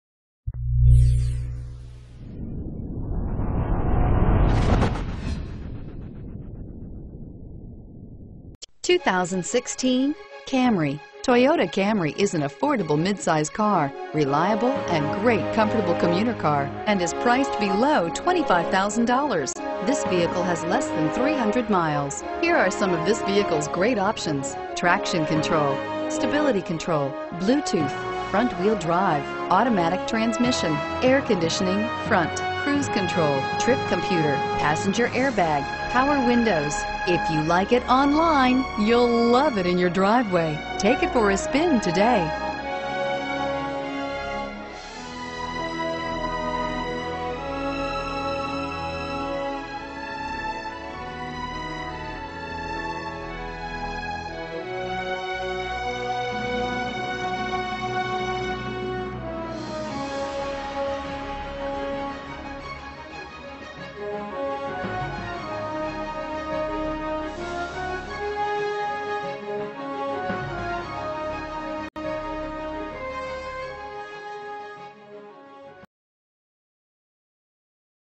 2016 Toyota Camry Kalamazoo MI A8476
2016 Toyota Camry LE

Metro Toyota
5850 Stadium Drive

Don't miss this 2016 Toyota Camry. It's equipped with Automatic transmission and features a Gray exterior. With 272 miles, You'll want to take this  home. Make a great choice today.